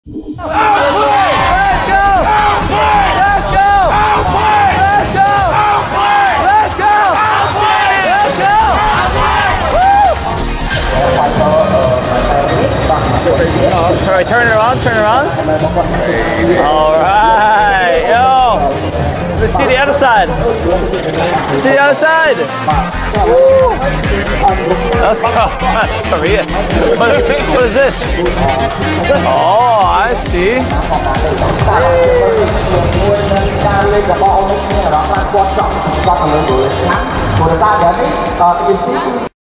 Thank you for your unwavering support! | MSC 2023 | Mobile Legends: Bang Bang
From the stands to the global stage, Outplay's fans are everywhere! These loyal supporters are ready to cheer on their favorite team in their fight to represent North America. Thank you for your unwavering support. Let's make some noise for Team OP!